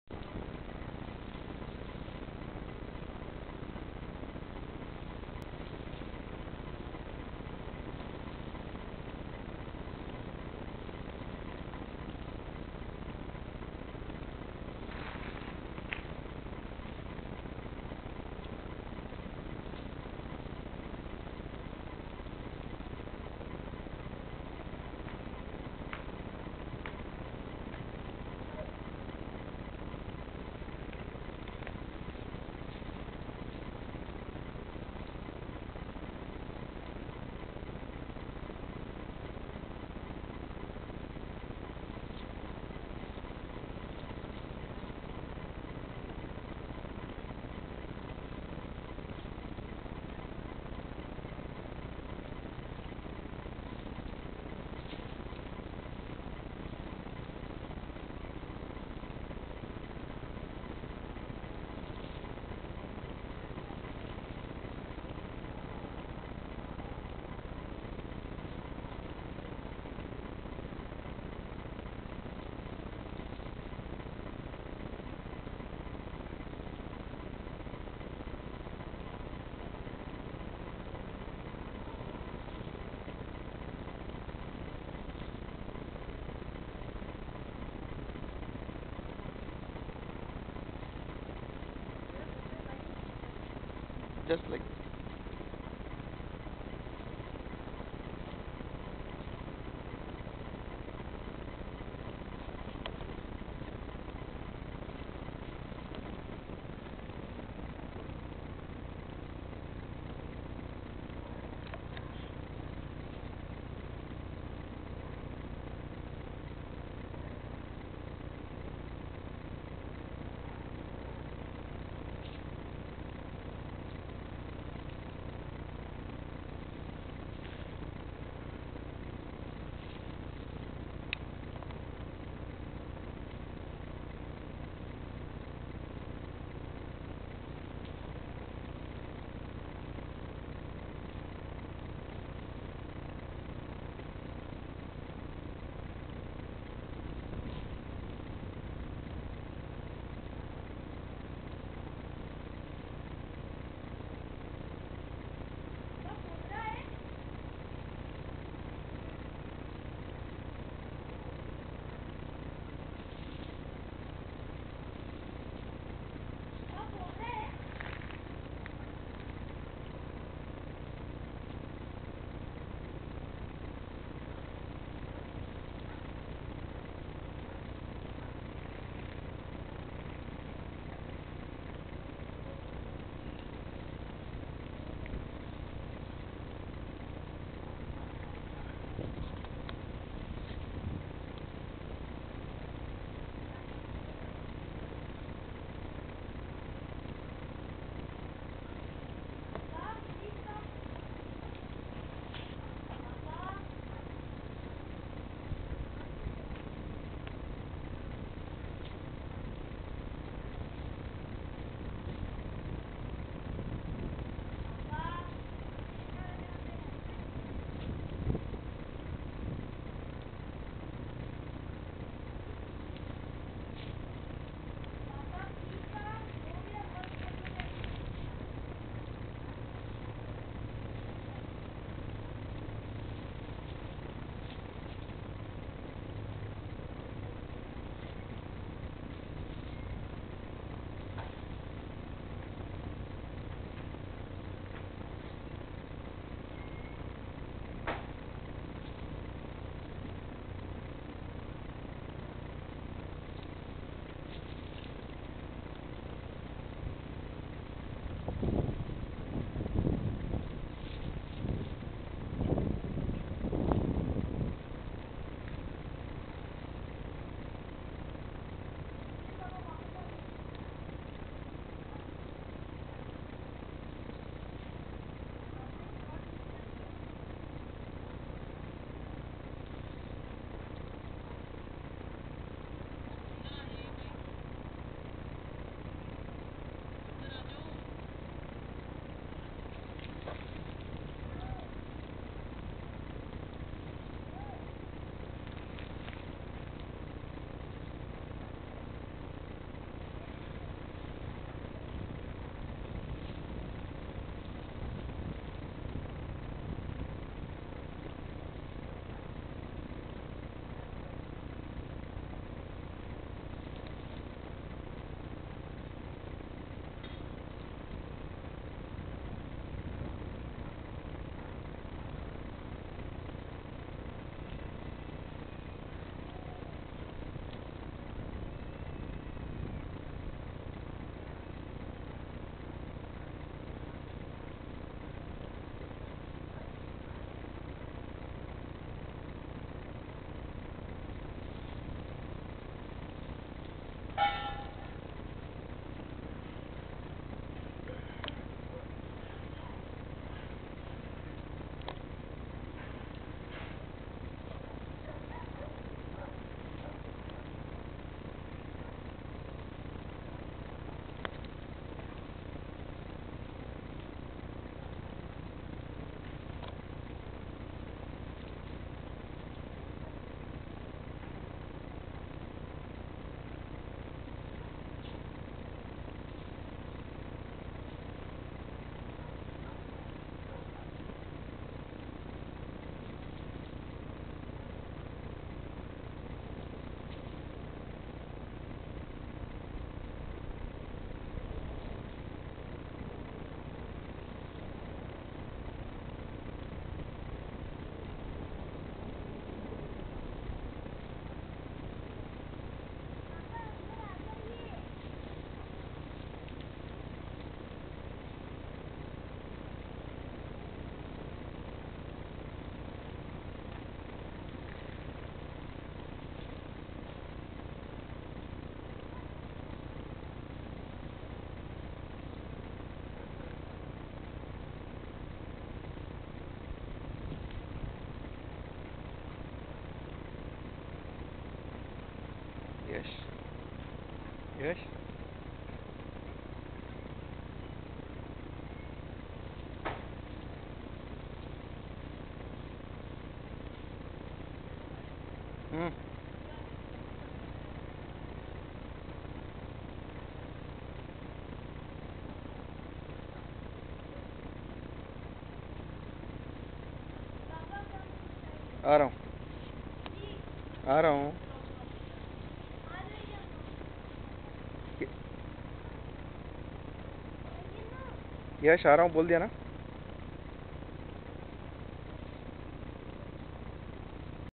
SNAKE FIGHT
This was a video shot behind my house at Belgaum in Karnataka in the year 2009.